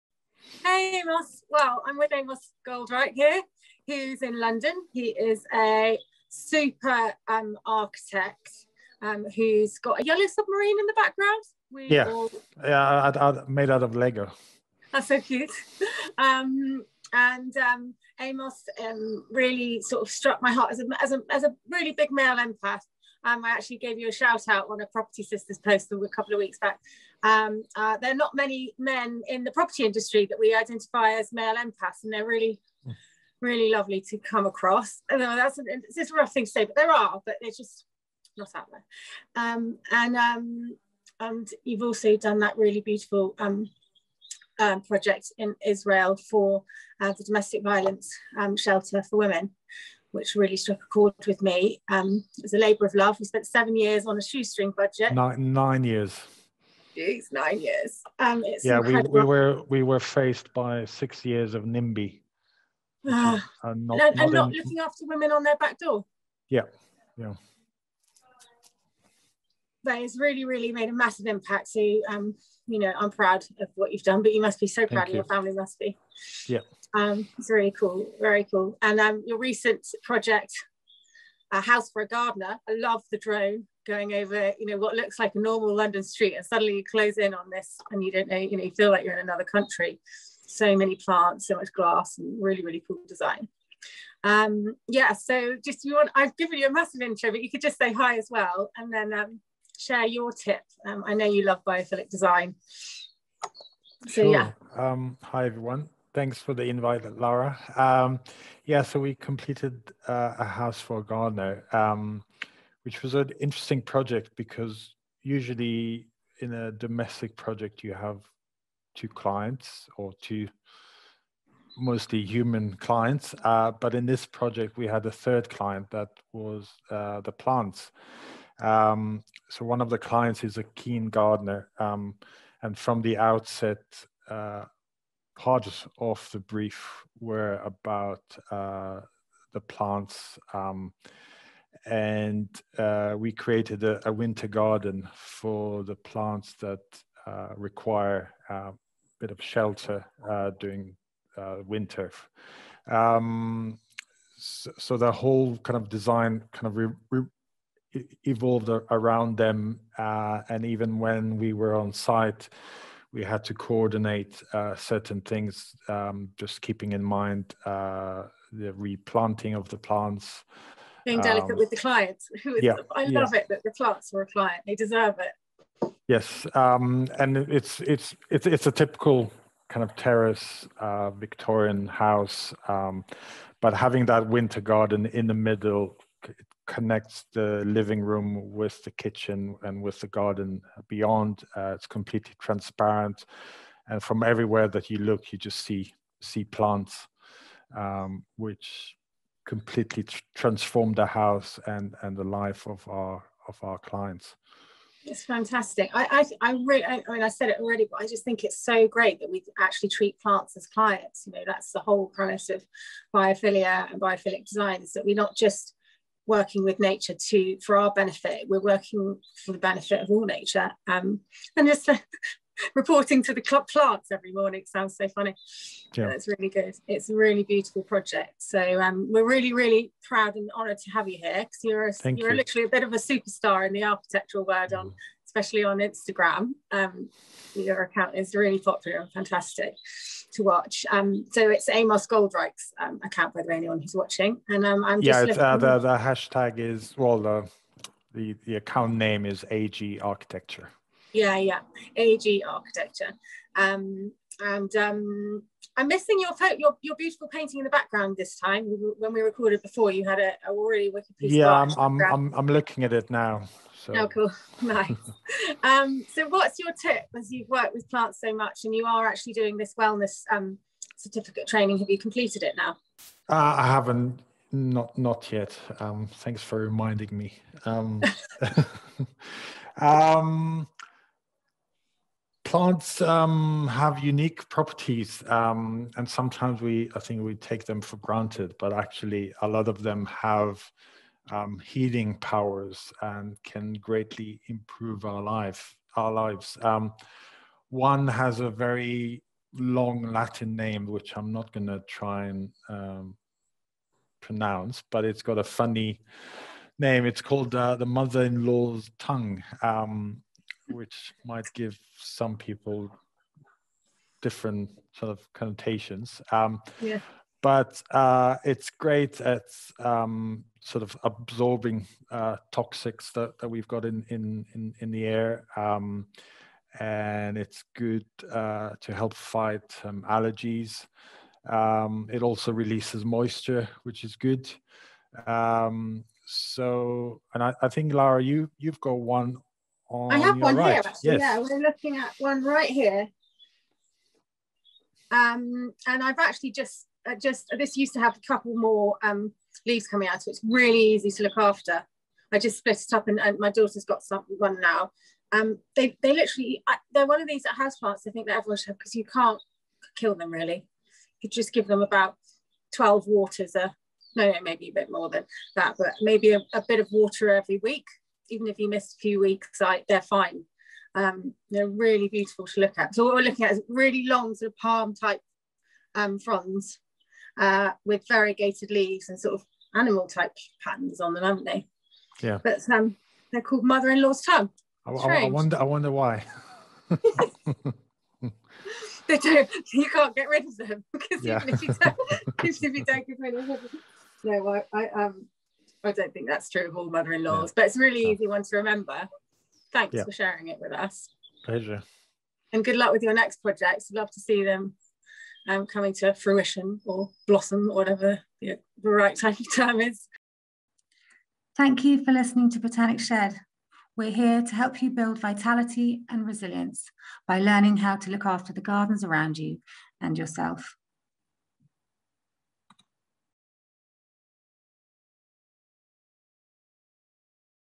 Nature Connection | Easy care house plant advice from architect Amos Goldriech
One of my hero's is an architect called Frank Llloyd Wright whose quote I recently found is basically my belief too “I believe in God, only I spell it Nature.”

Another great man in the architectural world is Amos Goldreich, who spent 9 years on a labour of love project building a shelter for domestic violence in Tel Aviv and most recently build a 'House for a Gardener' in London. As an architect he had three clients to report to on this project the couple and their plants. Exactly as it should be! The house will be featured in the May issue of Enki Magazine.

His quick Relate to Nature tip is to go out and buy one very oddly named plant for yourself... The Mother in Law’s Tongue!

The concept that humans have a biological need to connect with nature is called Biophilia, from the Greek, love of life and the living world. We love nature because we have learned to love the things that support us and help us survive. We feel comfortable and safe in nature because it is where we have lived for most of our life. Basically, we are genetically determined to love the natural world. It is in our DNA.  Plants at home act like a bridge opening our senses and elevating our moods, not just because they are ‘nice to have’ but for several logical reason like the fact they remind us about self-care, looking at their fractyls reduces our cortisol levels.

Richard Taylor at the University of Oregon, in his study Human Physiological Responses to Fractals in Nature and Art: Stress-Reduction, found that “people are 'hard-wired' to respond to a specific form of fractal found in nature, one that reduces stress levels by up to 60%.

Funnily enough though we haven’t been focusing on this in main stream house design for eons. Luckily now, as the correlation between mental health and nature is getting more coverage so are more and more designers and architects  bringing biophilic design into their work.

His 'House for a Gardener' project is described here: "A much-loved garden was central to our design of this side and rear extension to a Victorian mid-terrace house in the Stroud Green Conservation Area. The project for a couple, one a keen gardener, arranges a sequence of living spaces around the garden and a new plant-filled courtyard that gives constant connection to greenery on the tight urban site. Oak-clad beams spanning the expansive kitchen and dining area set up a rhythm that draws the eye out towards the garden, and a large bay window forms a seating nook above a fish pond."